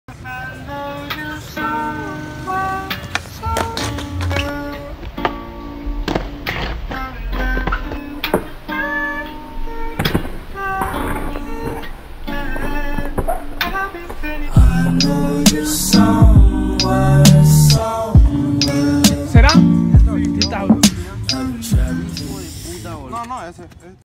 Mi único hall of meat (me rompo la pierna haciendo skateboarding)
Alv :v